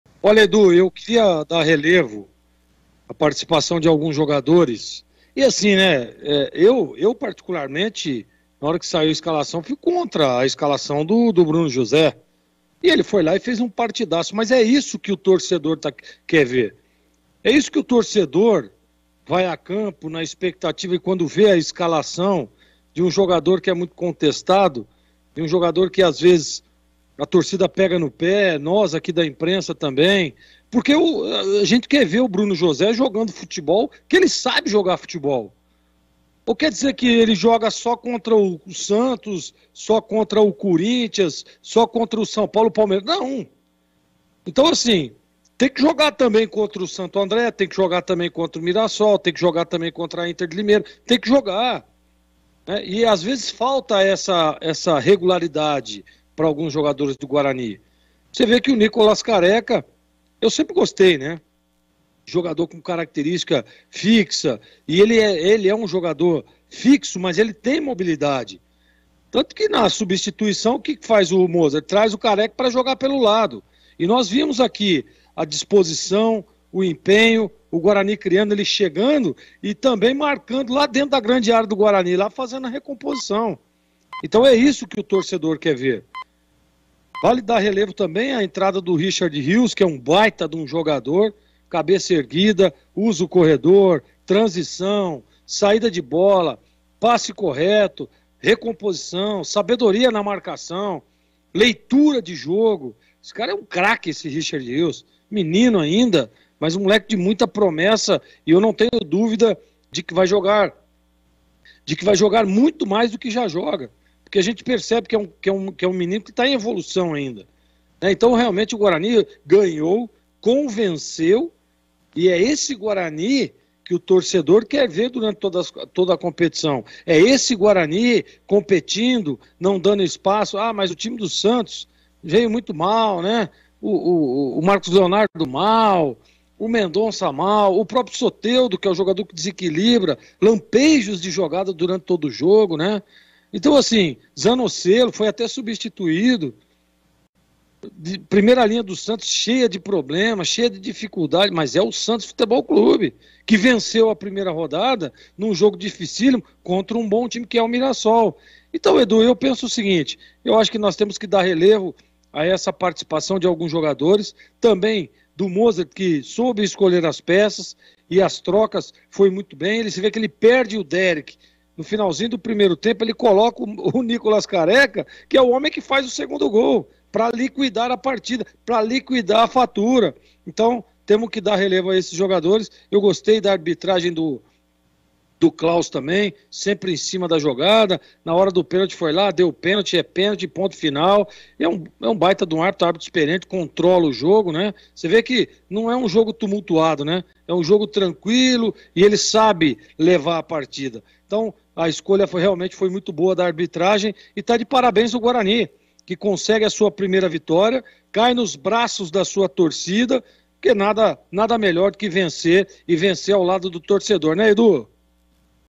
Gleguer: "Guarani ganhou e convenceu; Mozart soube escolher as peças" - 18/01/2023
Comentário final de Gleguer Zorzin sobre a vitória do Guarani sobre o Santos em transmissão pela Rádio Bandeirantes de Campinas - 18/01/2023